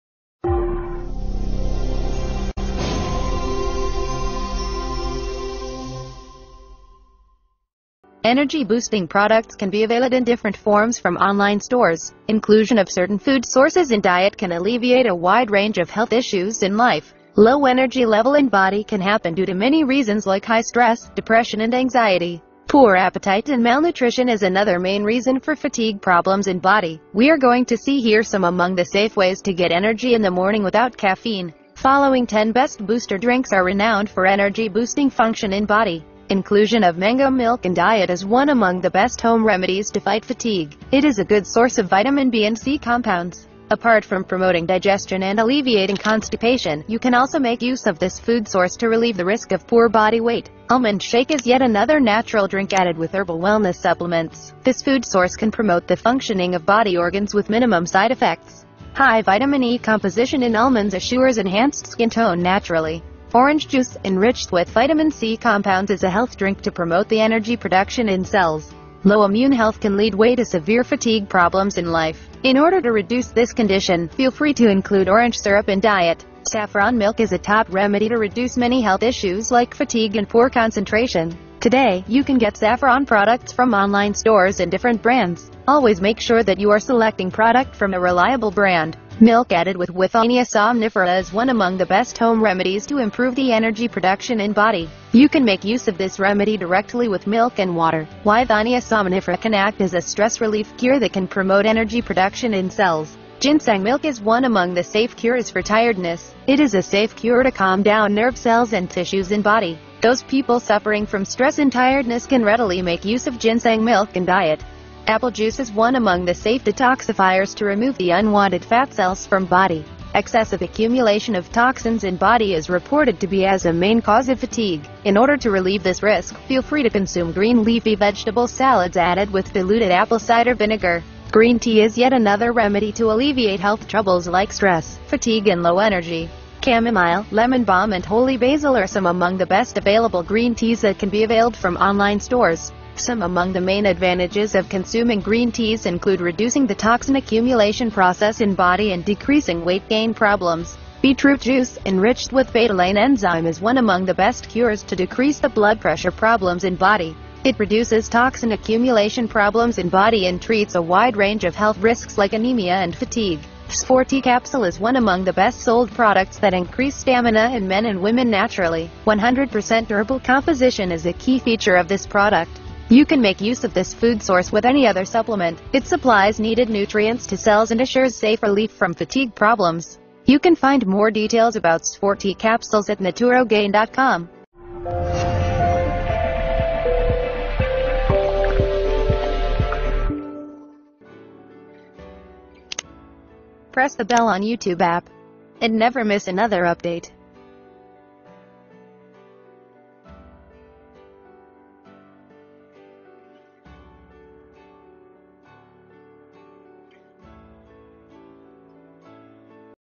9 Best Booster Drinks to Get Energy in the Morning without Caffeine
Best Booster Drinks to Get Energy in the Morning

In this health video, we have discussed 9 best booster drinks that help you to get energy in the morning without caffeine.

After waking up not feel refreshed? Are you searching for the best drink that keeps you the energetic whole day without caffeine?

Your search ends here in this video we have discussed the best energy drinks that keep you healthy, alert, and refreshing the whole day. You can easily get these supplements in your kitchen.

Let’s take a brief introduction about what are these drinks:

1. Beetroot Juice
2. Green Tea
3. Apple Juice
4. Ginseng Milk
5. Milk Added With Withania Somnifera
6. Saffron Milk
7. Orange Juice
8. Almond Shake
9. Mango Milk

Along with these drinks if you take Sfoorti capsules you will get more benefit. After regular usage of these supplements, you feel energetic, alert, healthy, and happy.

Thanks for giving your valuable time watching the video. Have a healthy and fit body!